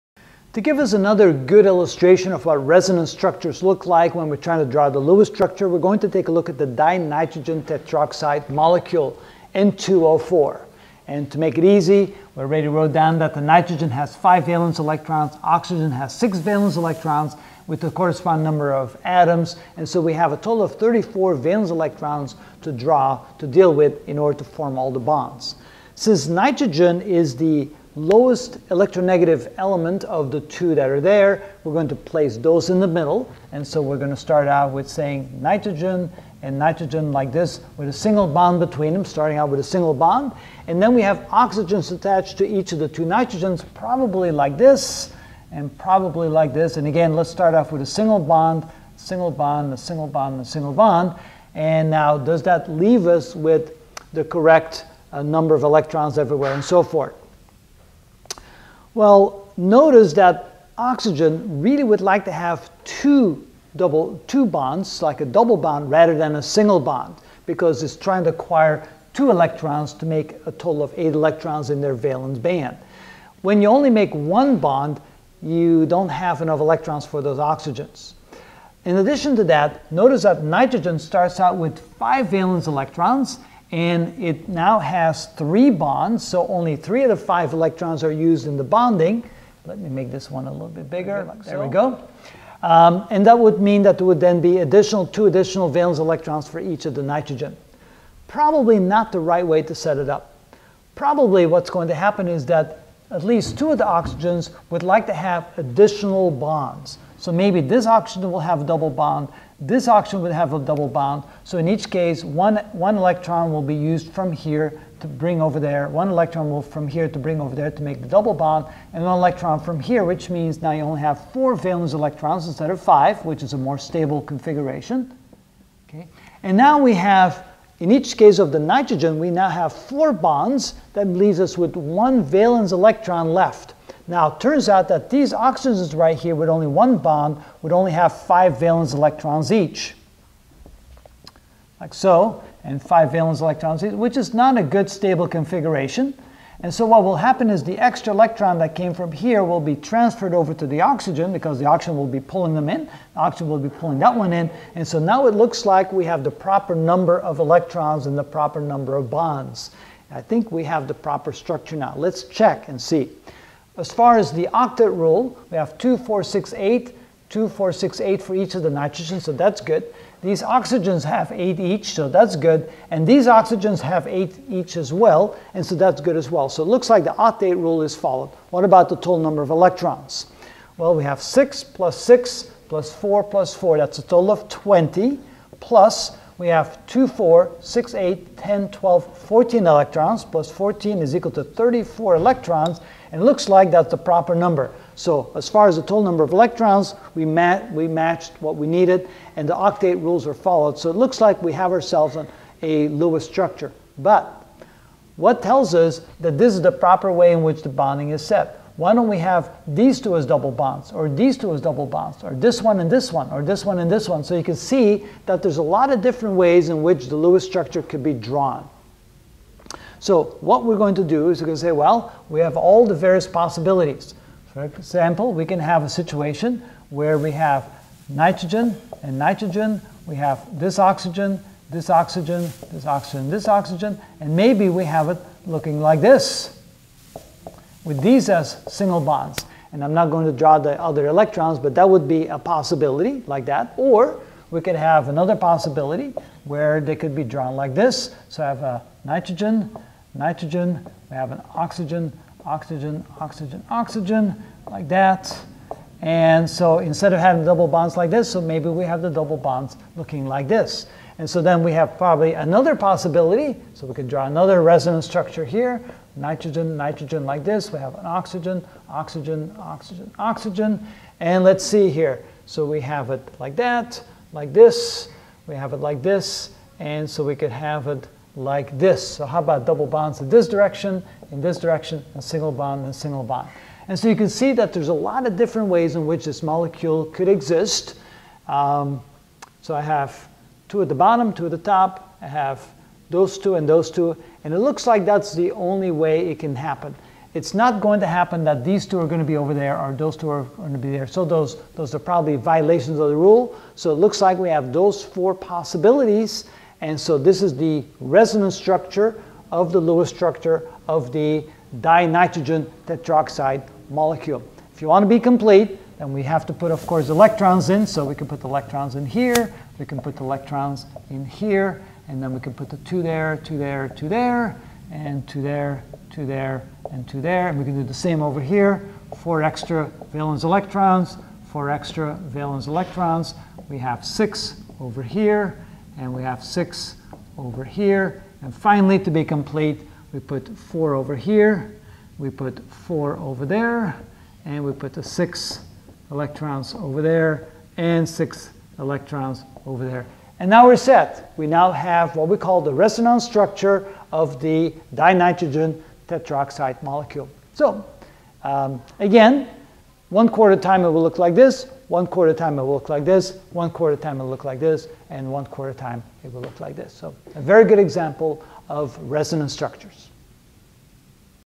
Chemistry - Chemical Bonding (28 of 35) Lewis Structures (Resonance) - Dinitrogen Tetroxide
In this video I will show the Lewis structure for the resonance structures of dinitrogen tetroxide, N2O4.